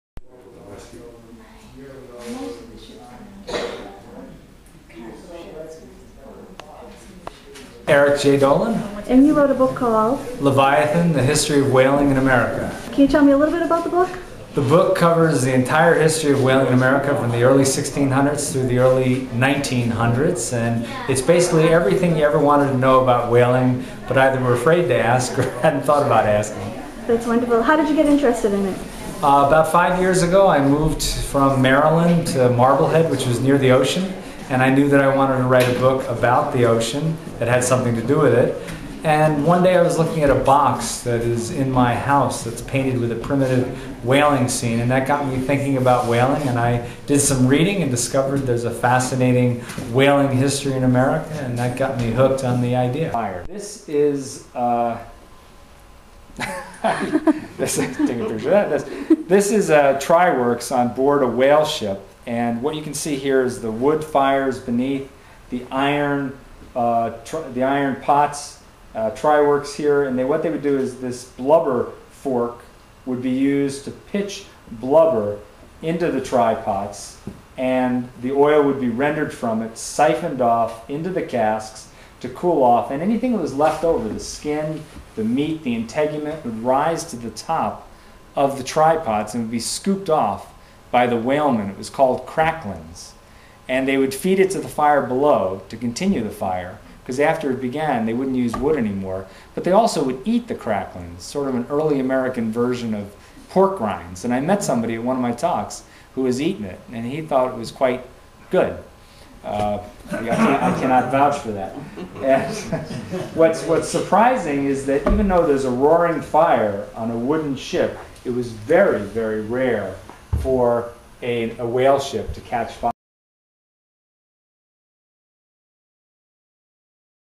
"Leviathan" Whaling Author Eric Jay Dolin in Hull
On Sunday afternoon, Cathleen took in a discussion at the Lifesaving Museum by "Leviathan" author Eric Jay Dolin. The book is about whaling.